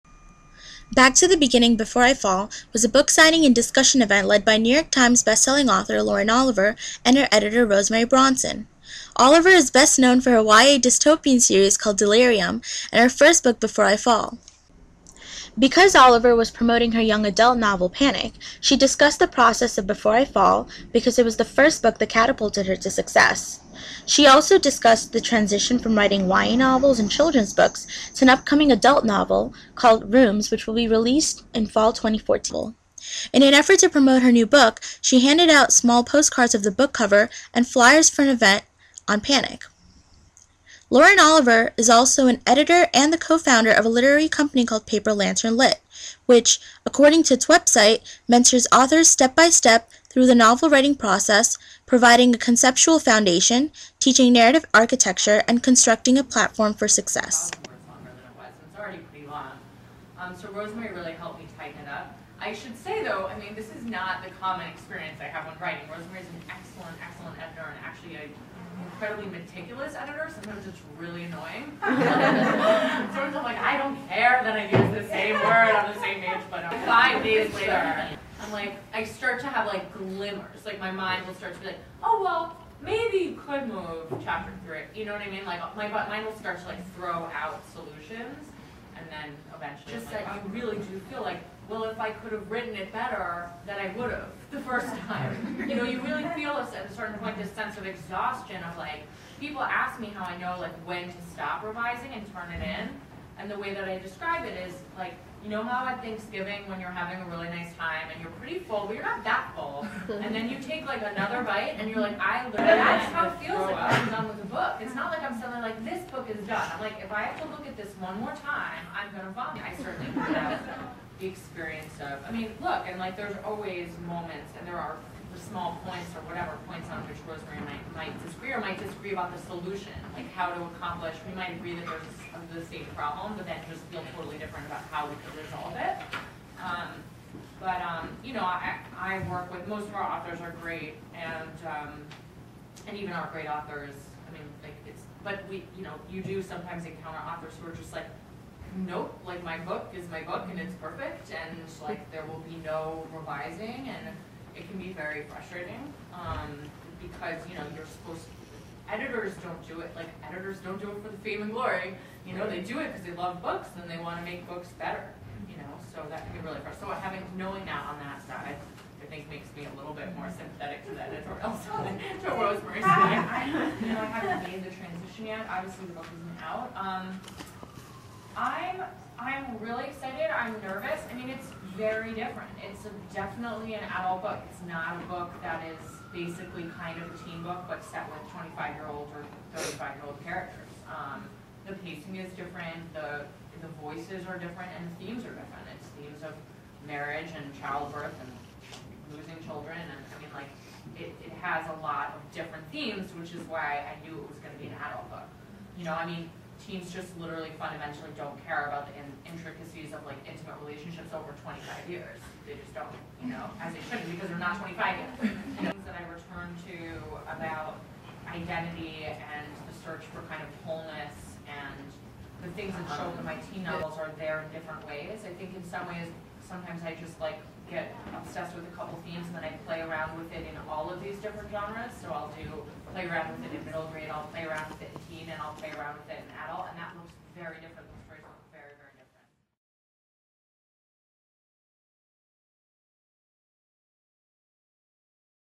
Back to the Beginning Before I Fall event with Author Lauren Oliver
Back to the Beginning: Before I Fall event with Lauren Oliver and her editor at Books of Wonder on February 12, 2014. At the event, Lauren talked about the editing process of Before I Fall, the difficulties she, as a writer, faced when editing her book, and being an editor at Paper Lantern Lit.

Event flier courtesy of Books of Wonder
Paper Lantern Lit photo courtesy of Paper Lantern Lit.